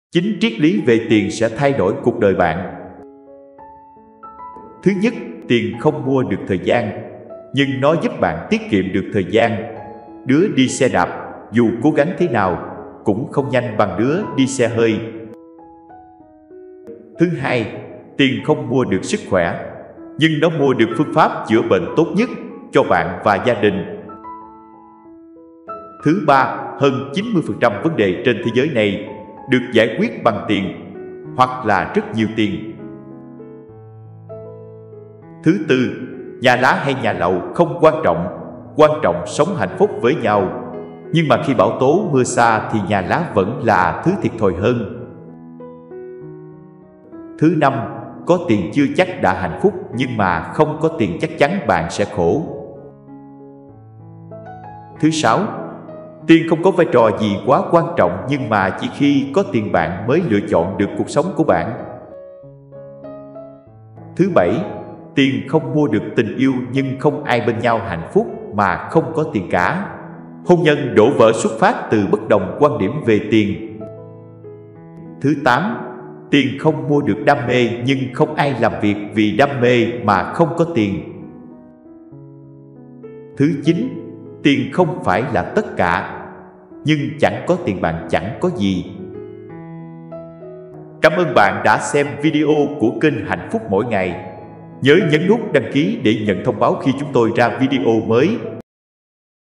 9 Triết Lý Về Tiền Sẽ Làm Thay Đổi Cuộc Đời Bạn | Hạnh Phúc Mỗi Ngày
9 Triết Lý Về Tiền Sẽ Làm Thay Đổi Cuộc Đời Bạn | Hạnh Phúc Mỗi Ngày
💌Thứ 1: Tiền không mua được thời gian, nhưng nó giúp bạn tiết kiệm
💌Thứ 2: Tiền không mua được sức khoẻ
💌Thứ 3: Hơn 90% vấn đề trên thế giới này được giải quyết bằng
💌Thứ 4: Nhà lá hay nhà lầu không quan trọng,quan trọng sống hạnh phúc
💌Thứ 5:  Không có tiền chắc chắn đời bạn sẽ khổ
💌Thứ 6: Tiền có ảnh hưởng đến quyết định chọn nghề nghiệp và lập gia đình của bạn
💌Thứ 7: Tiền không mua được tình yêu
💌Thứ 8: Đam mê không có tiền là đam mê bỏ đi
💌Thứ 9: Tiền không phải là tất cả.
© Bản Quyền Thuộc Về Hạnh Phúc Mỗi Ngày
© Copyright by Hạnh Phúc Mỗi Ngày ☞ “Do not Reup”